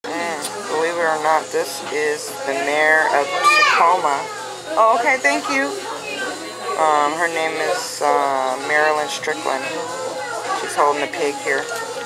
City of Tacoma Has African American Woman Mayor Marilyn Strickland But You Would Never Know
About Mayor Strickland
Marilyn Strickland was sworn in as Mayor of Tacoma in January 2010, and previously served as a city council member. Born in Seoul, Strickland is a graduate of the University of Washington and holds a Master's Degree in Business Administration from Clark-Atlanta University. She and her husband Patrick Erwin live in the downtown.